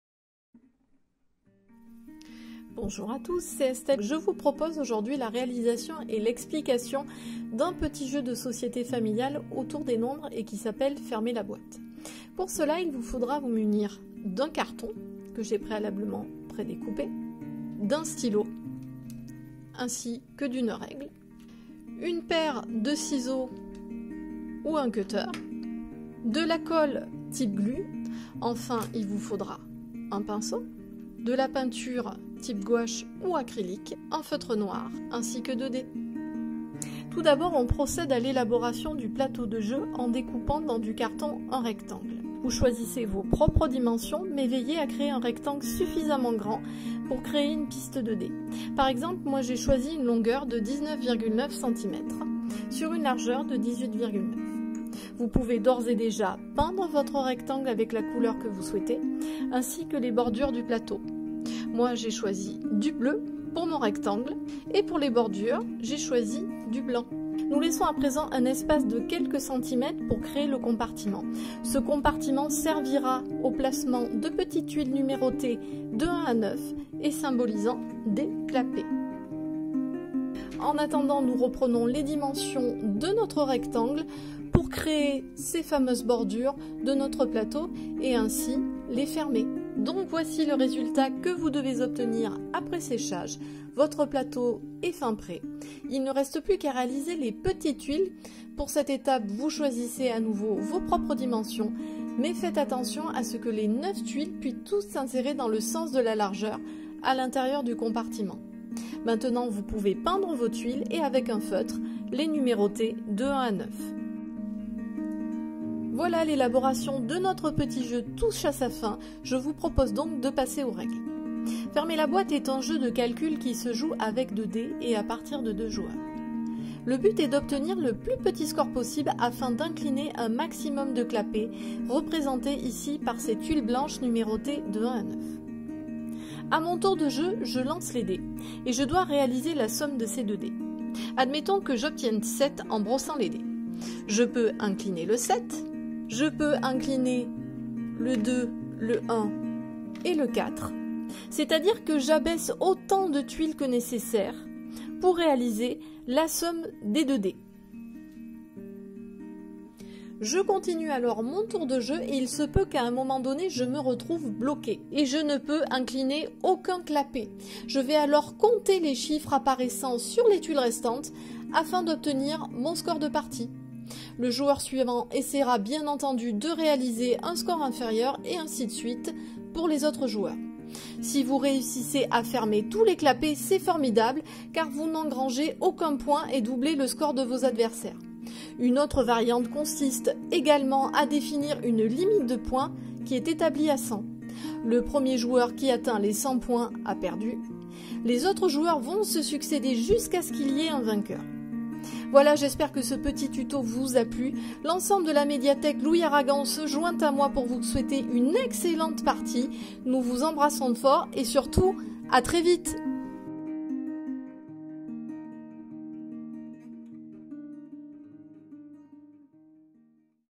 Tuto "Fermez la boîte"
Estelle vous propose de fabriquer un jeu de calcul, pour apprendre en s'amusant !